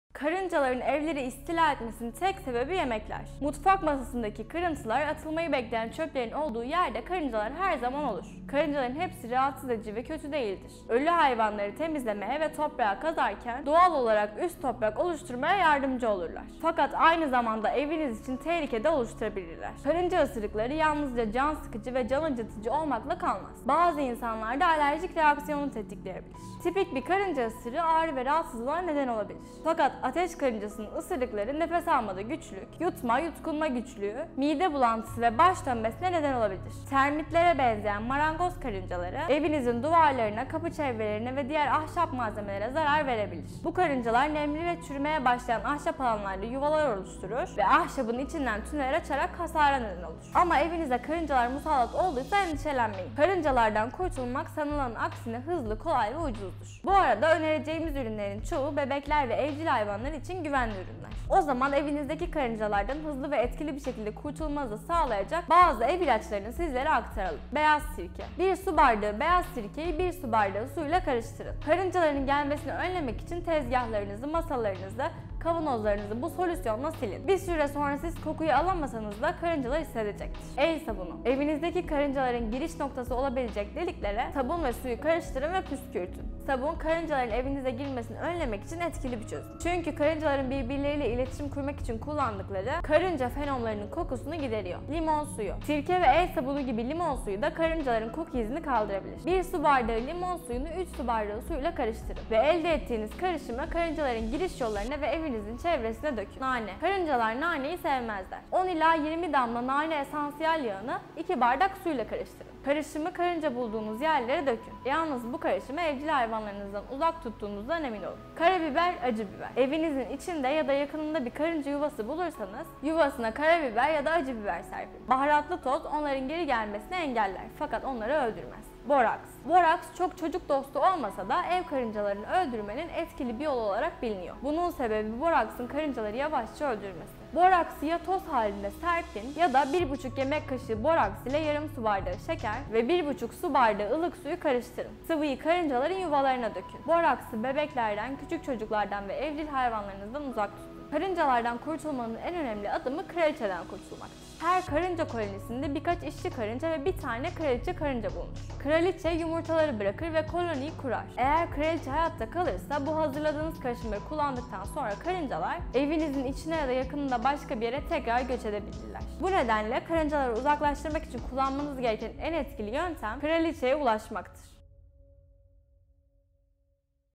Karıncalardan kurtulmanın yolları!
Karıncaların evleri istila etmesinin tek sebebi yemekler. Karıncaların hepsi kötü ve rahatsız edici değildir. Fakat aynı zamanda eviniz için tehlike de oluşturabilirler. Bu videomuzda evinizdeki karıncalardan hızlı ve başarılı bir şekilde kurtulmanız için bazı ev ilaçlarını size aktardık. İyi seyirler…